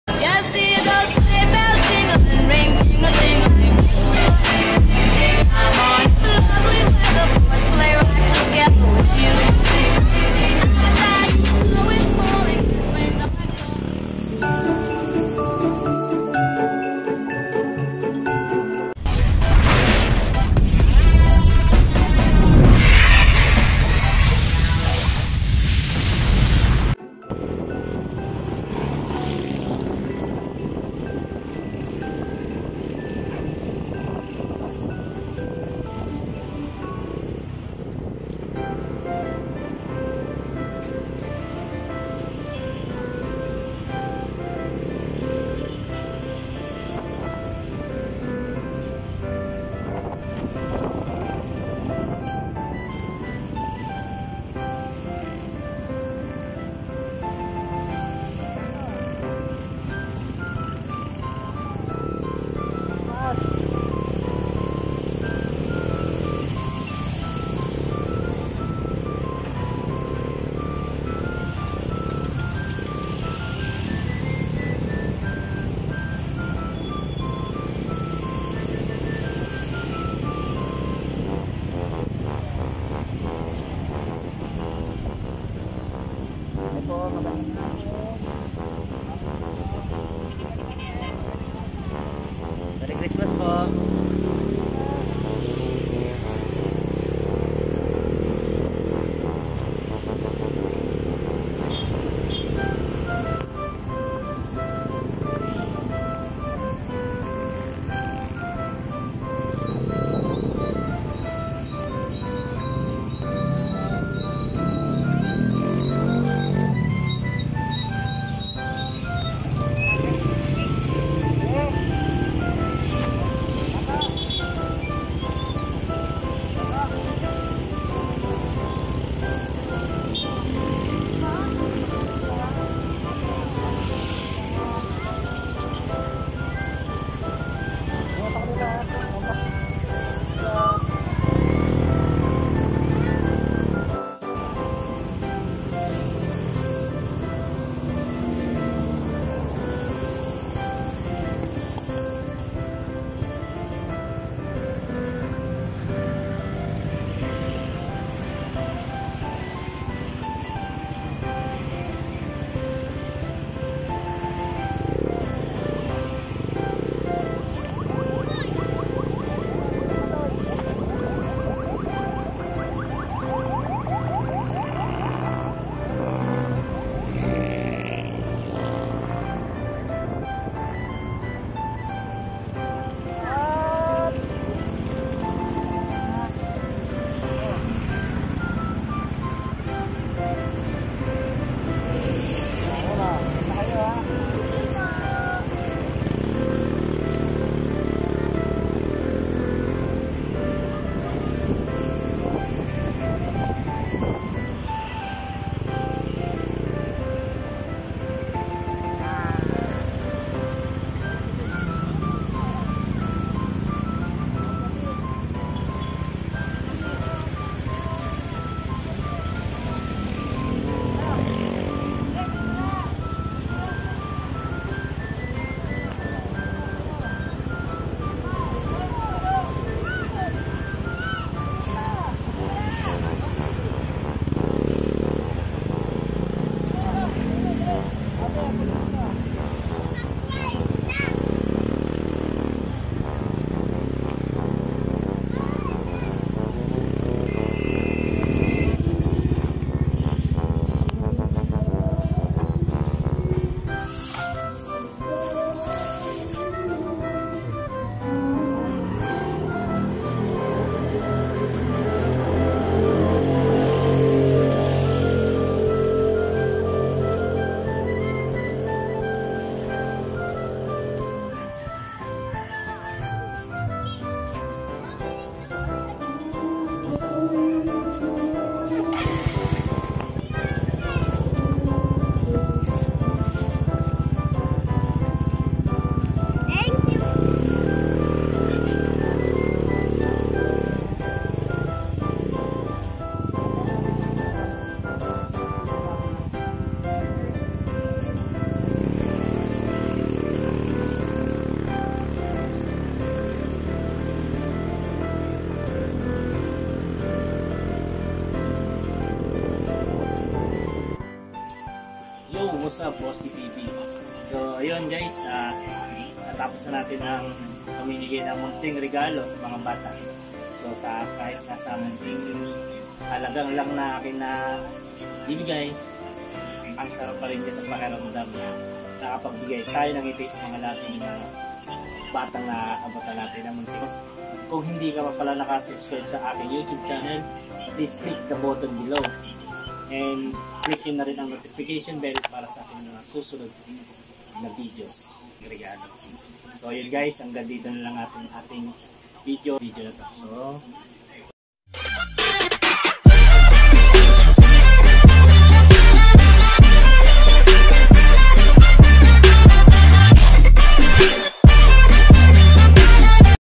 Give Love Part 2: Munting regalo ating handog
Yoww wazzup guizz
Part 2 na tayo ng ating bday celebration🎉🎂
Sana sa maliit nating relago ay
Napangiti natin ang ating mga naabutan ng munting regalo
Salamat sana magtuloy tuloy pa ang blessing
Na dumating satin mga bossG

Late upload: Last december pa yan guyz